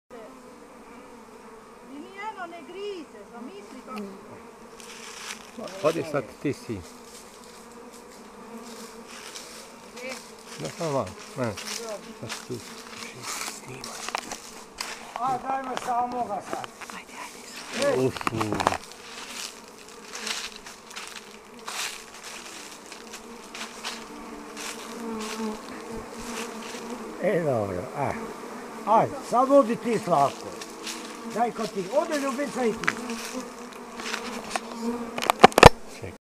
Pčelar Ivan Curek iz Podravine 2
Ivan Curek pčelar iz podravskog sela Miholjanec selekcijom več 40 godina odabire miroljubljive pčele, koje nikome od posjetitelja neće nanjeti ubod.